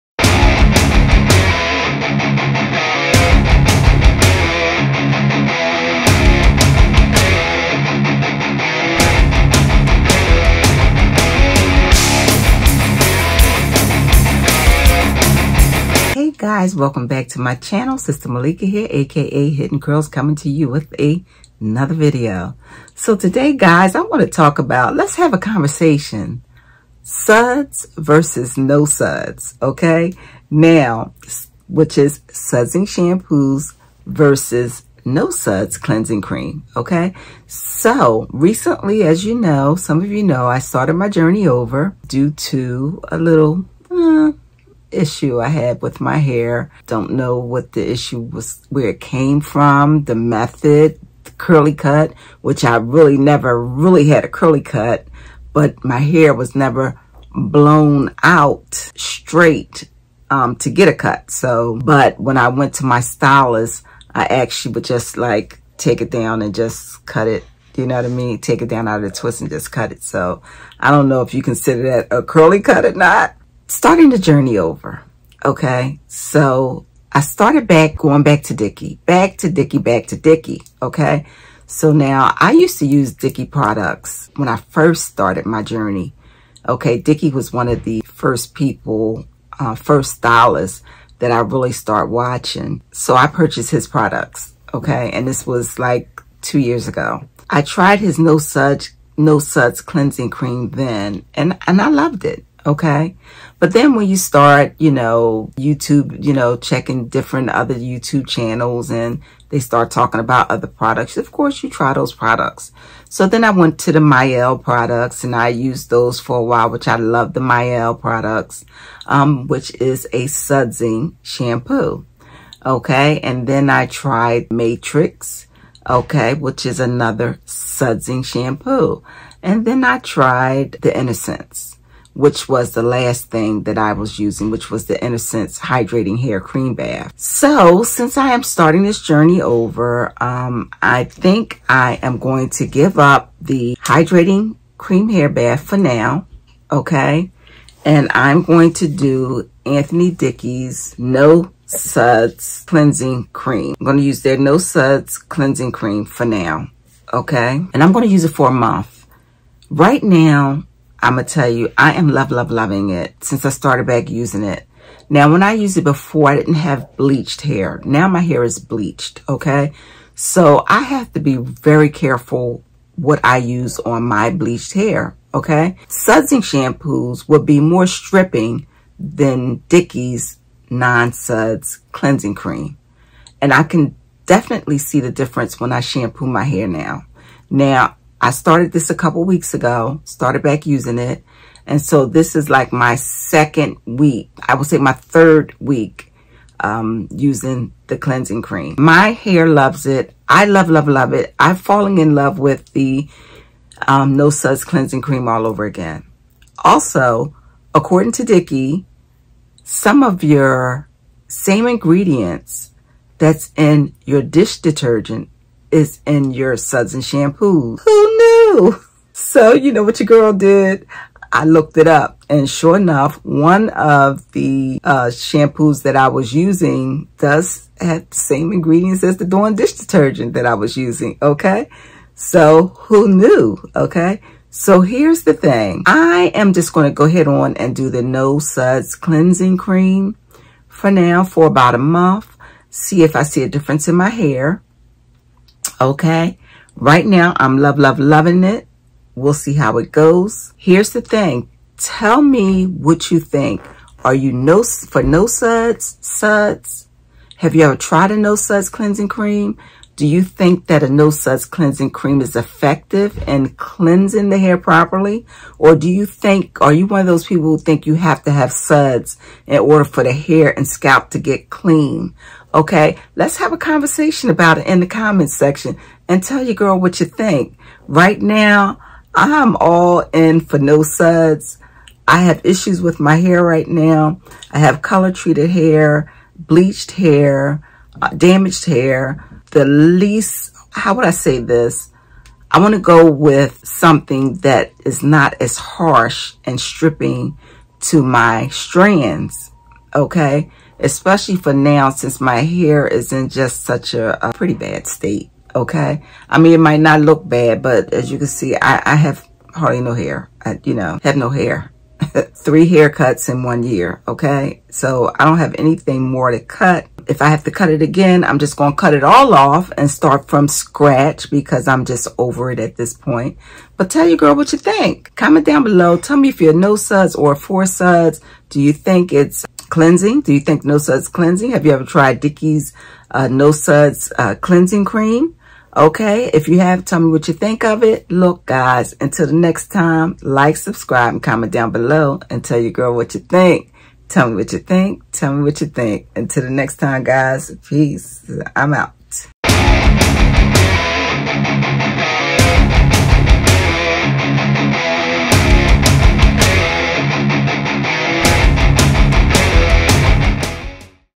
Is Anthony Dickey NO SUDS shampoo good for natural hair
Hi guys today I share my experience and why I have been using Anthony Dickey no suds shampoo. I hope you enjoy this video.